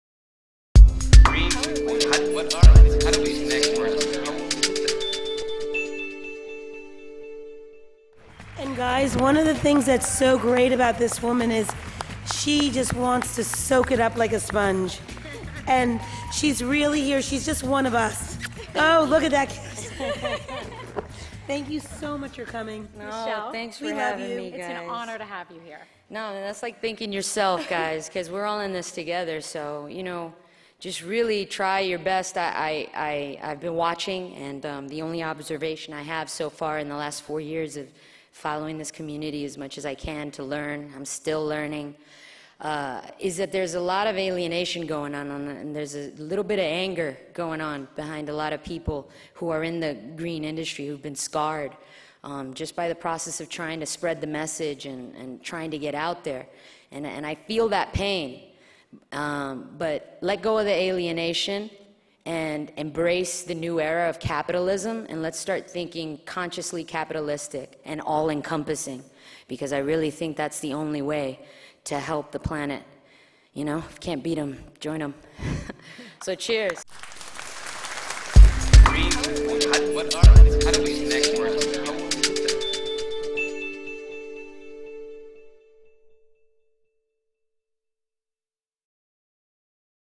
Ac"CLIMATE"izing Society to the "New Economy"
The non-profit industrial complex (with 350.org, Avaaz, Pupose Inc. etc. at the helm) utilizes celebrity and behavioral change tactics to spread their ideas through the western "culture" - it's important to understand what ideas they wish to take hold.

The Michelle Rodriguez award acceptance video which sums of the purpose of "opportunity green":

"Let go of the alienation and embrace the new era of capitalism and let's start thinking consciously capitalististic and all encompassing."

Welcome to your new economy. The same as the old economy.

This changes nothing.